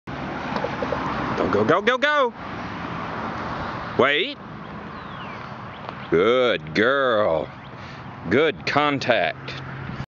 Swedish Vallhund Puppy Agility Distance Training - Dog Walk
Five month old Hermione is practicing her dogwalk with gradually increasing height and send out distance. We train for an automatic sit in the contact zone.